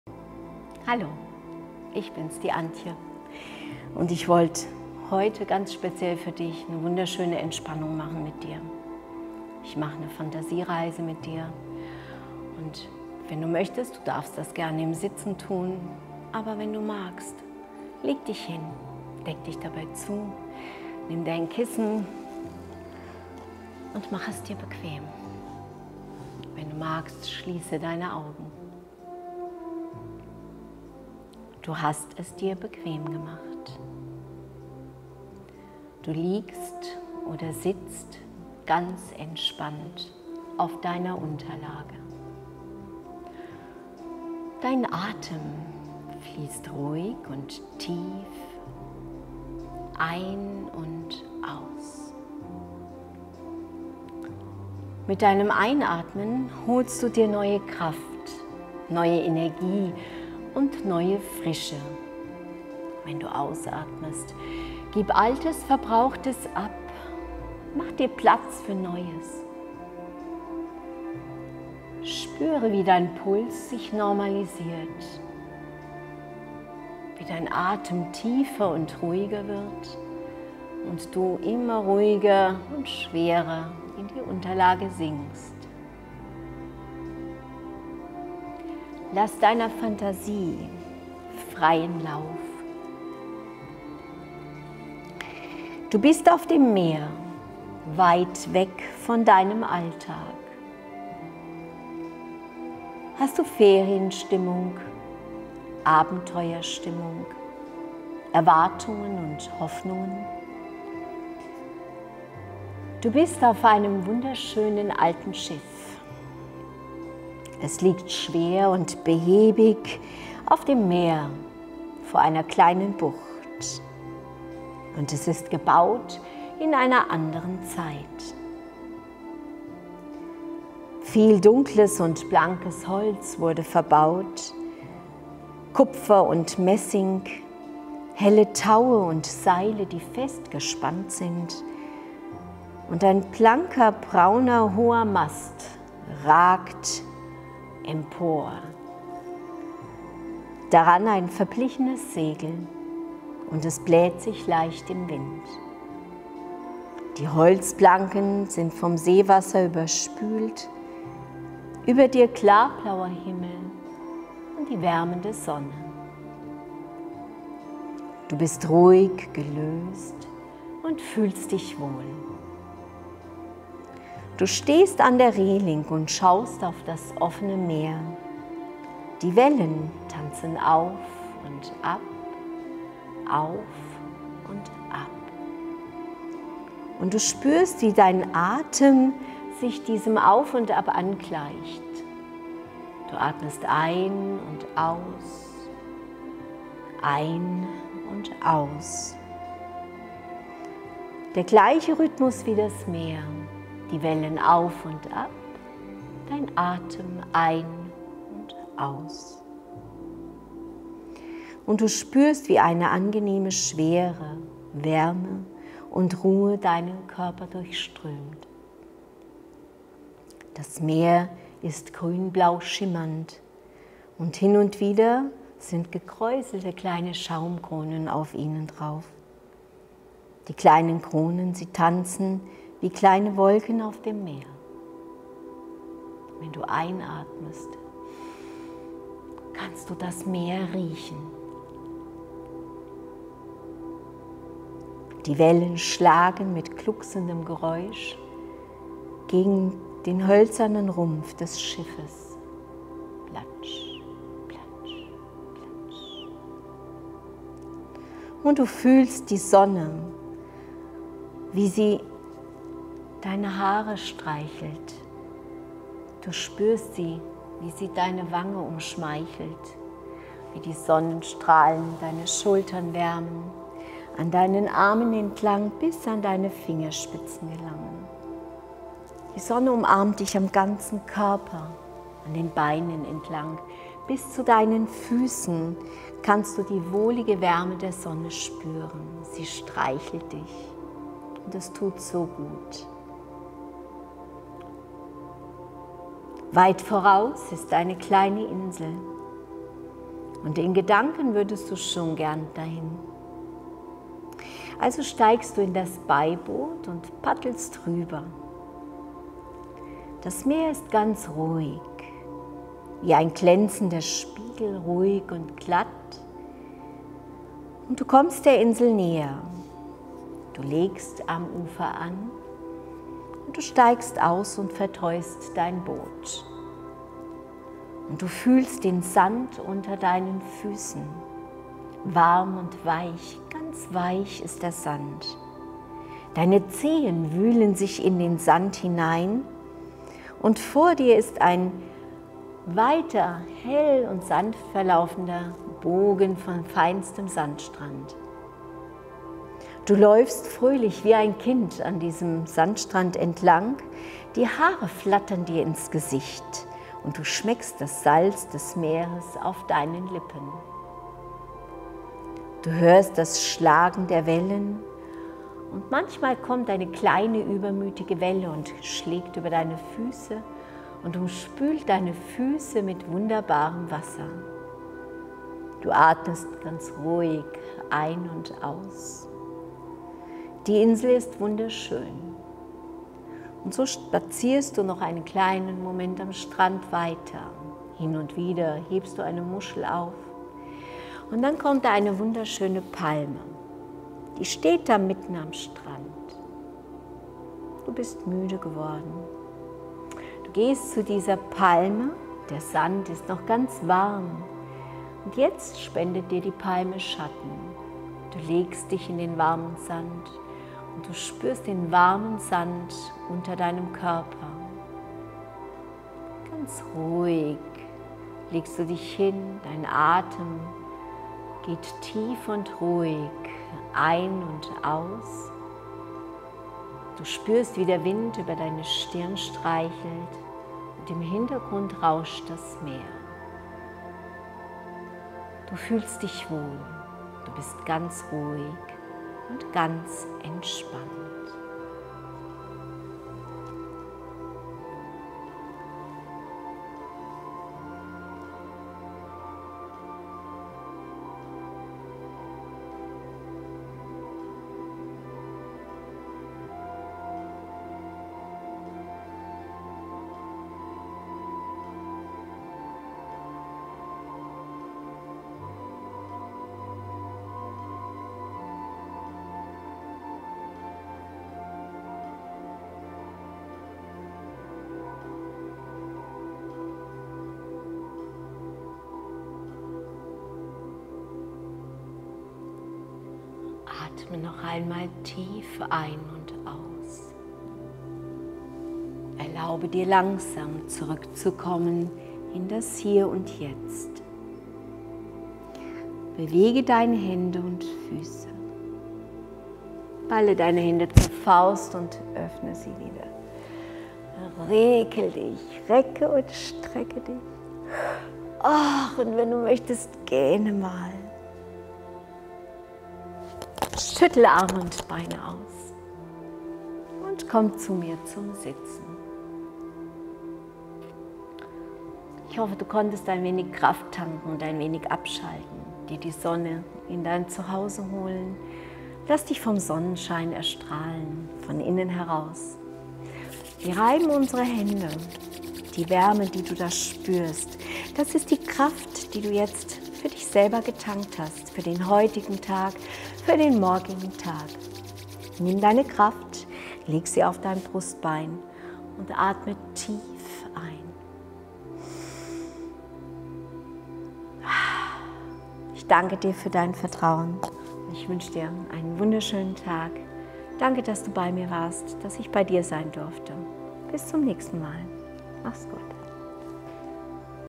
Entspannungsübungen mit Antje | fitnessclub lifeline
Wie wäre es mit ein wenig Entspannung nach einem langen Arbeitstag?

Tue dir was gutes und atme gemeinsam mit Antje mal wieder so richtig durch.